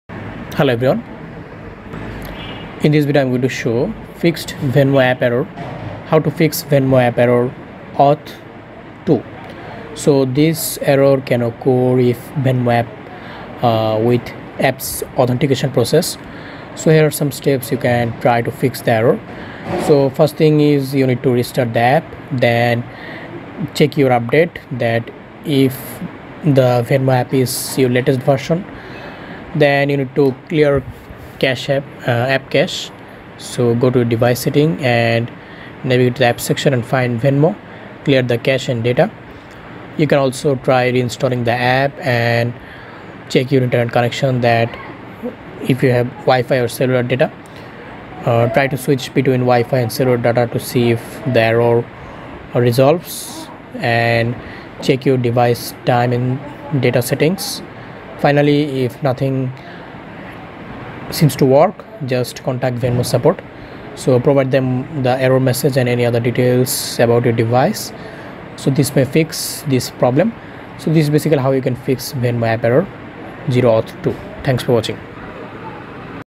Fixed Venmo App Error: How To Fix Venmo App Error OAuth2.0 | Easy Guide 2024
Fixed Venmo App Error: How To Fix Venmo App Error OAuth2.0

If you're facing a Venmo app error, this easy guide will help you fix it. Learn how to resolve the OAuth2.0 issue and transfer money smoothly!

Would like to get a brand deal with us?